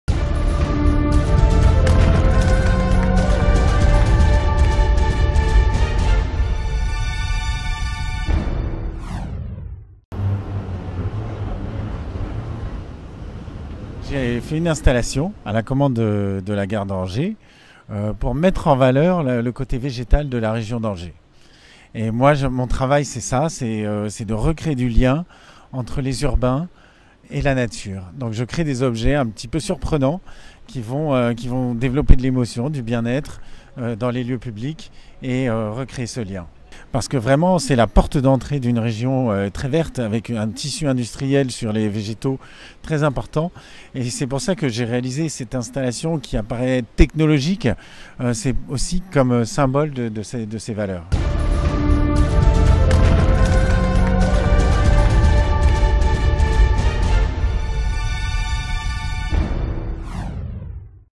Inauguration de "La Fôret suspendue à la gare SNCF d'Angers
Depuis quelques jours, des compositions végétales aériennes sont présentes dans le hall de la gare Saint-Laud d’Angers. 5 arbres entourés d’un anneau lumineux ont été installés. Son concepteur, Alexis TRICOIRE, veut redynamiser le lieu par ses créations futuristes.

Des compositions végétales futuristes ont investi le hall de la gare d’Angers. Ce projet « Gare Jardin » a été réalisée sur mesure par le studio de design végétal Alexis TRICOIRE. En levant la tête vers le ciel, 5 arbres satellisées par des anneaux lumineux en acier inoxydable flottent à 5 mètres de hauteur le long de la façade vitrée de la gare. Il faut compter 1 tonne pour le poids d’un satellite. Le paysage est d’autant plus beau la nuit tombée, donnant un aspect zen et dynamique de la gare d’Angers à ces 15 000 passagers quotidien. Sur le parvis extérieur, 2 autres satellites vont atterrir (voir photo). Le premier est déjà en place depuis ce matin. Pour le second, il faudra attendre jeudi prochain. Ces deux satellites non suspendus sont là pour offrir aux voyageurs des espaces de confort pour s’asseoir, se relaxer en attendant son train.

Ces compositions végétales disposent du label Fleurs de France et viennent confirmer le titre de la ville la plus verte de France donné à Angers lors du palmarès 2017 de l’Union Nationale des Entreprises du Paysage et Hortis. « C’est mon rêve de voir des forêts voler. À travers ce projet, je veux que l’on apporte un regard nouveau sur le végétal », explique le créateur Alexis TRICOIRE. « J’espère vraiment pouvoir donner une nouvelle identité à la gare d’Angers ».

Et si vous appréciez ses œuvres, rendez-vous au festival international des jardins de Chaumont sur Loire pour admirer d’autres projets d’Alexis TRICOIRE.